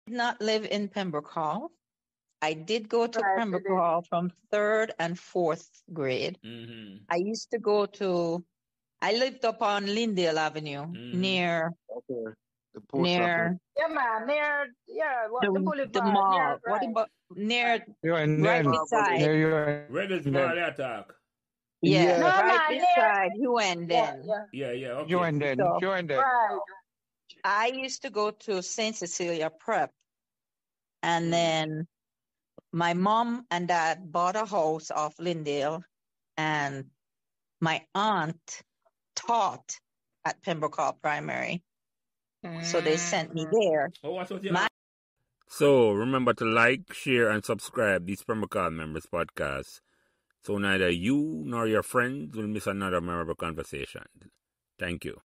JENNIFER MILLINGEN TRAILER # 1
Members of the Graduating Class of 1973 from Pembroke Hall Primary (Brian Walters, Jacque Watkins-Johnson, Nigel Joseph, Colleen Grubb, Dwight Henry, Audrey Martin Vishu Tolan and Jennifer Millingen share their memories of Pembroke Hall .